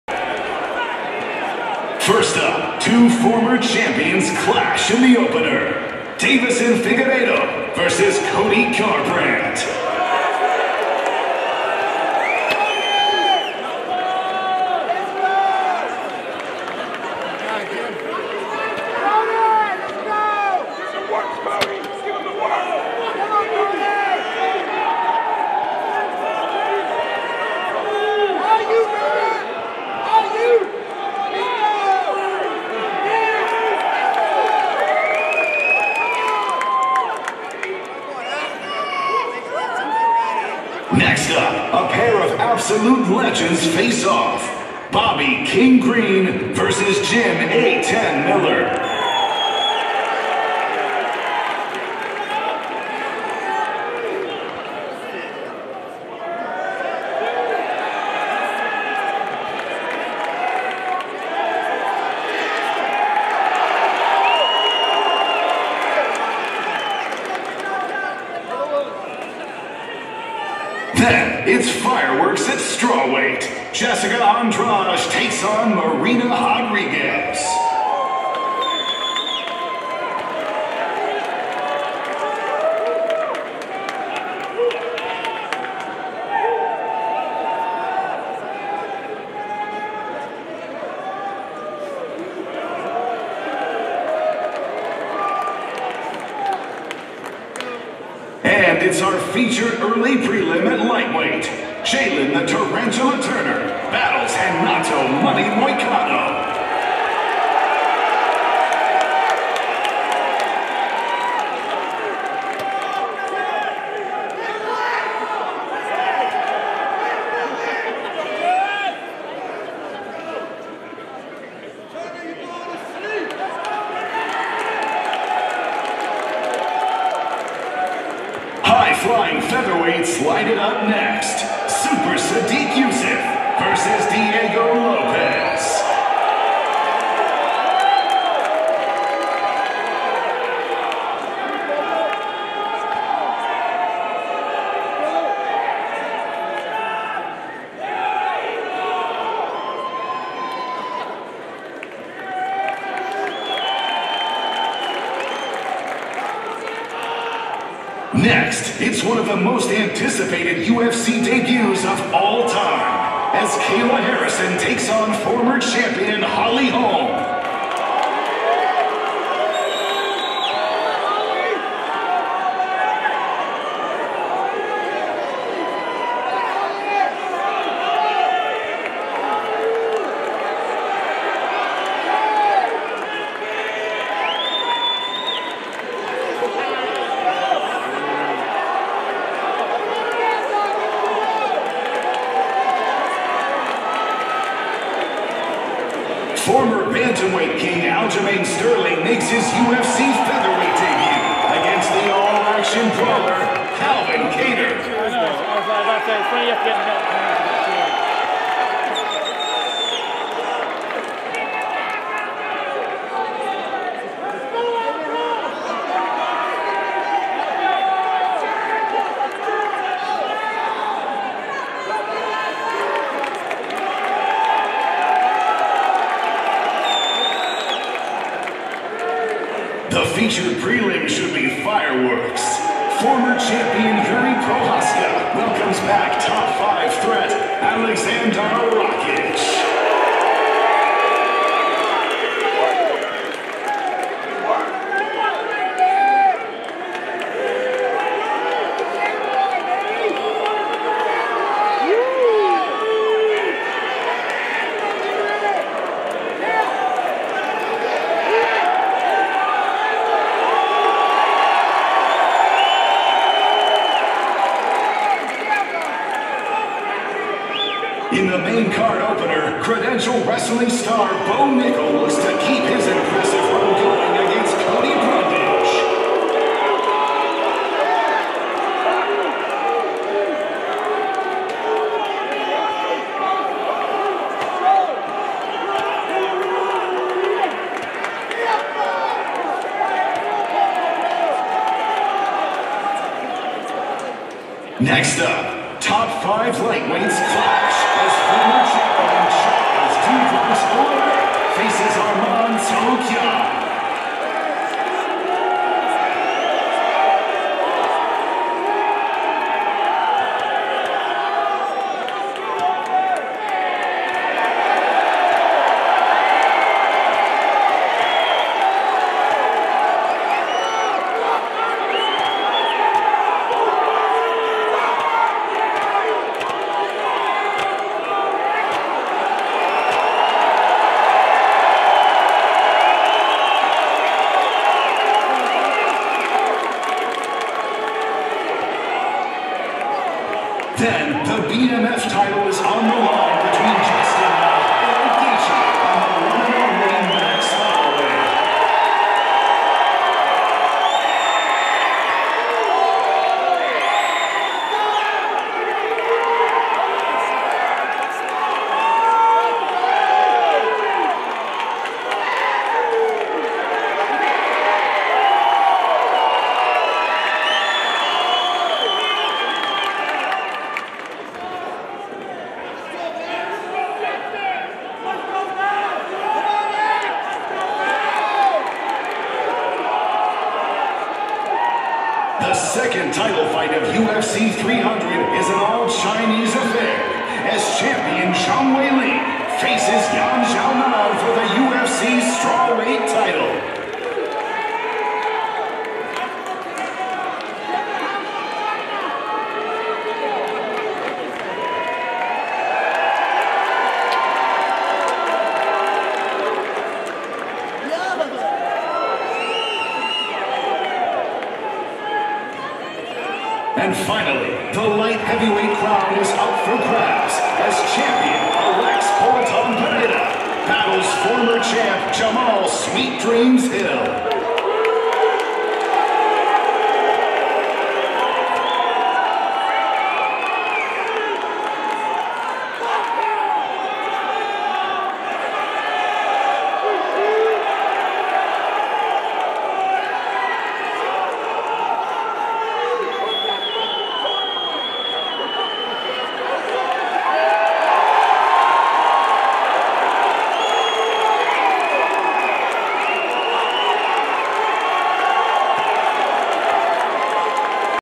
Alex Pereira vs. Jamahal Hill Face To Face | UFC 300
Alex Pereira and Jamahal Hill face off ahead of UFC 300.